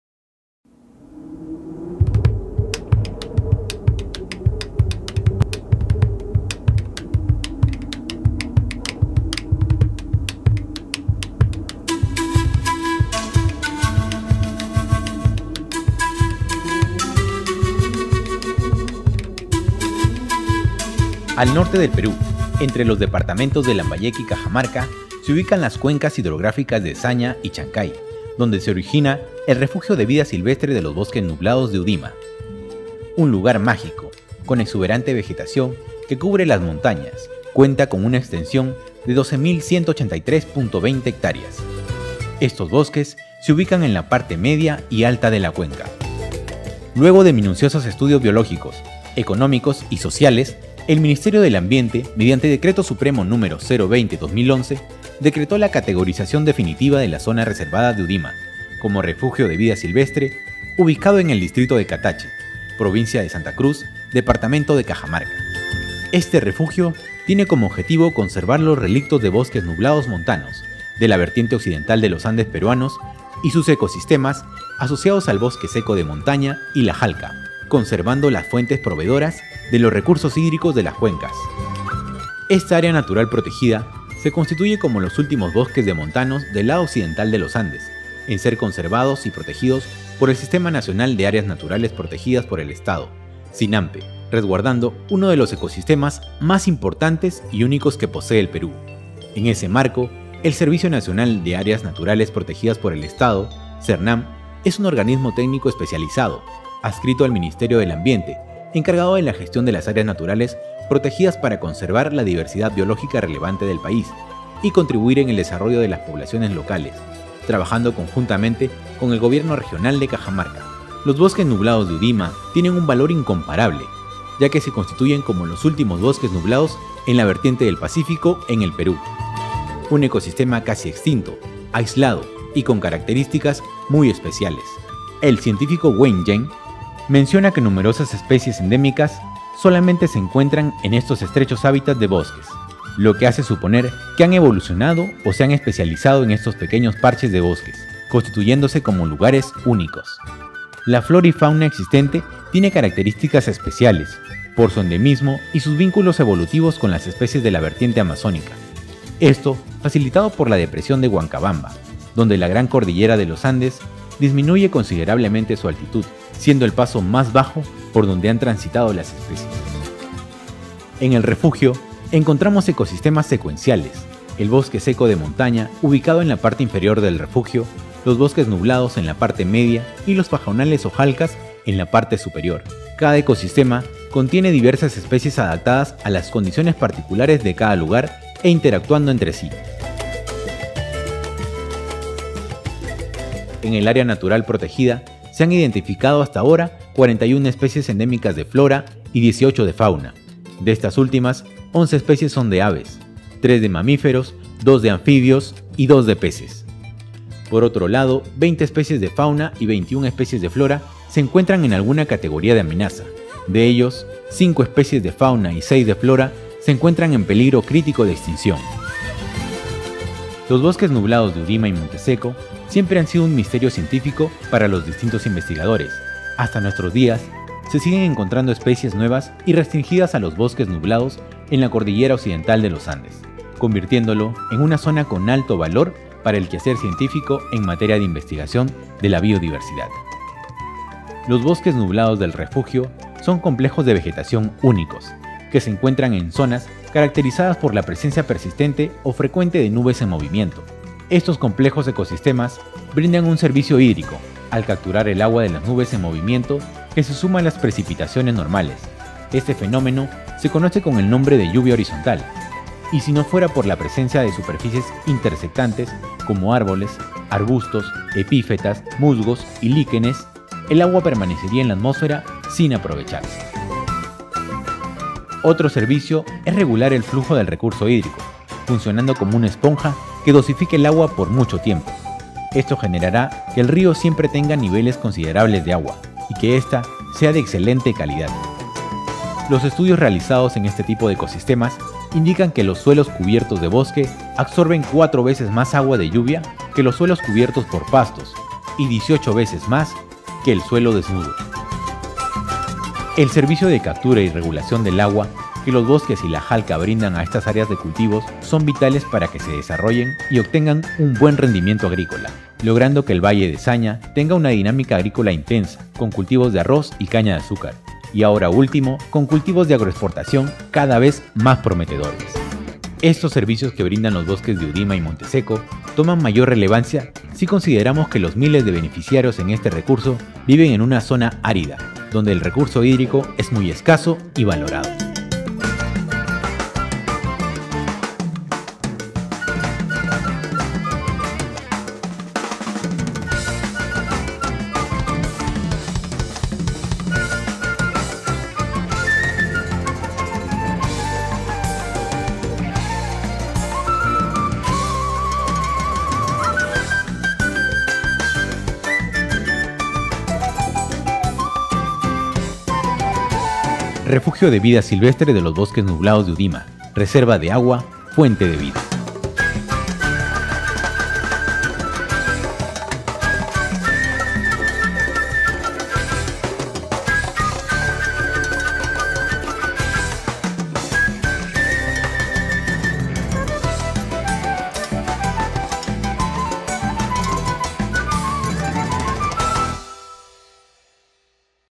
Spot promocional ” Refugio de vida silvestre: Bosques nublados de Udima”
La naturaleza nos ofrece lugares únicos y mágicos, como Udima, un refugio de vida silvestre que se merece lo mejor de nosotros por que nos brinda lo mejor de el.